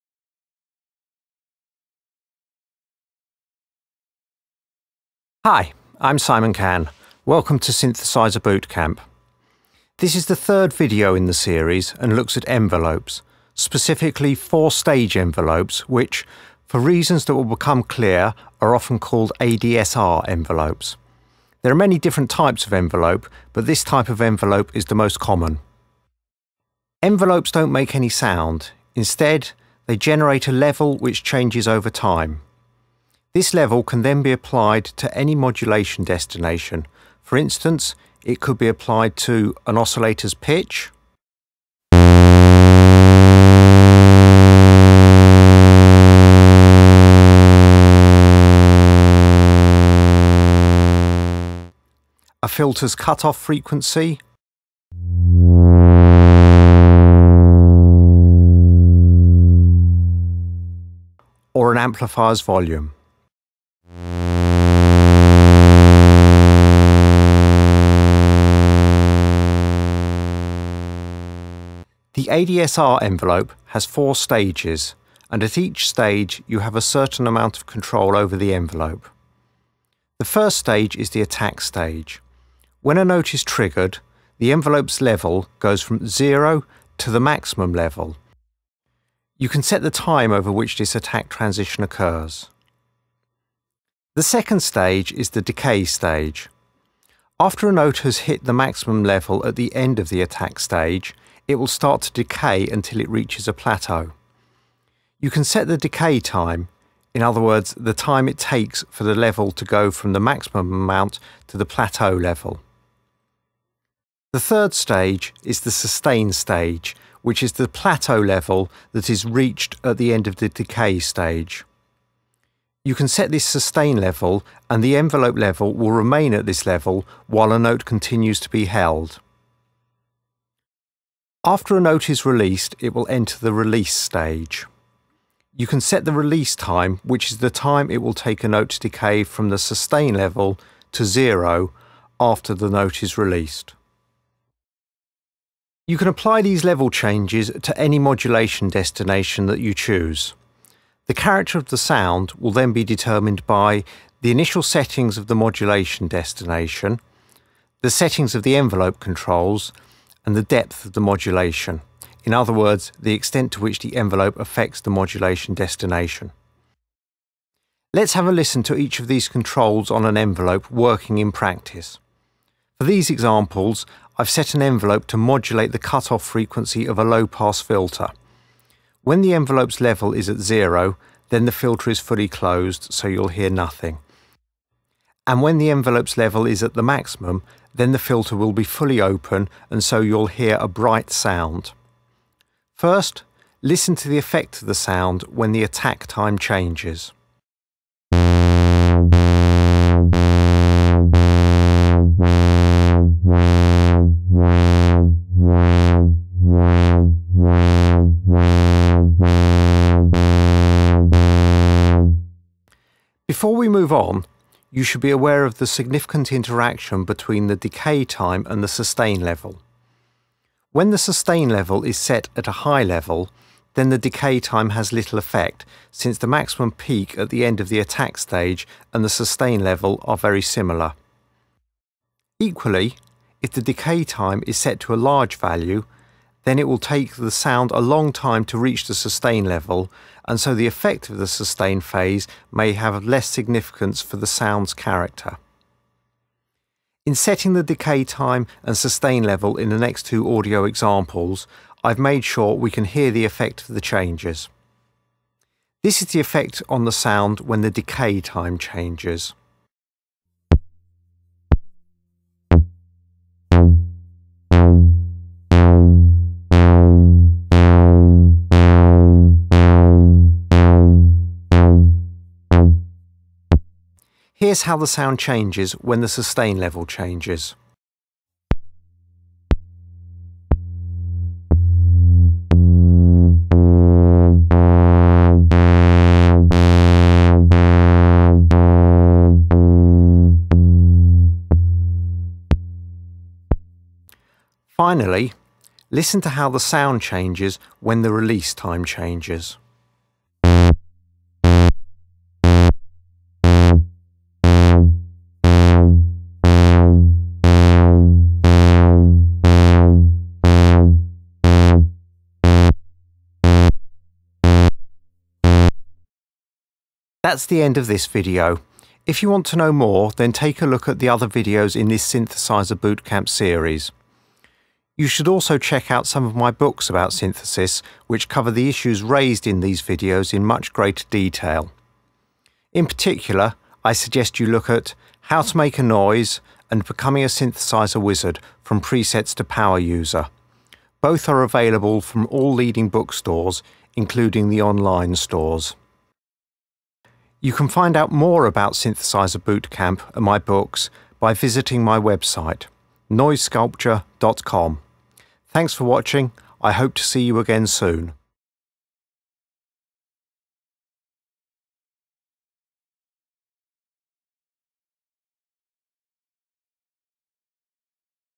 Synthesizer Boot Camp #3--Envelopes: Attack, Decay, Sustain, and Release
Synthesizer Boot Camp is a series of videos which looks at specific synthesizer techniques that can be applied in creating sounds. This video looks at using envelopes as a modulation source.

You can find out more about the series and check out to read more about synthesis.